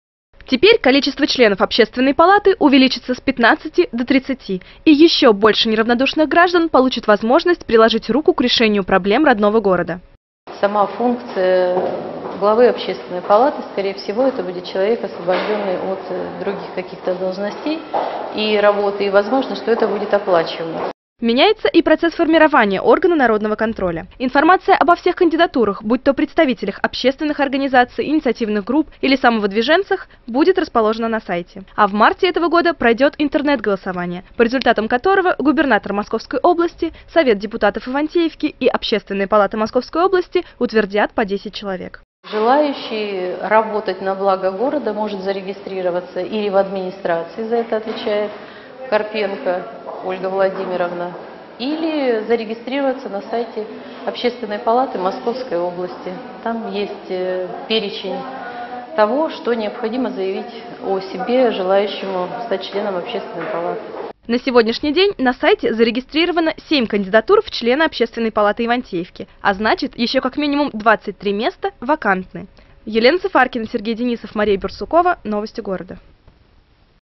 Реформирование общественной палаты в Ивантеевке. Эфир 28.02.14.
Преобразовать, нельзя оставить. Губернатор Московской области Андрей Воробьев однозначно расставил приоритеты развития Подмосковья и призвал реформировать все общественные палаты в регионе. Перезагрузка началась и в Ивантеевке.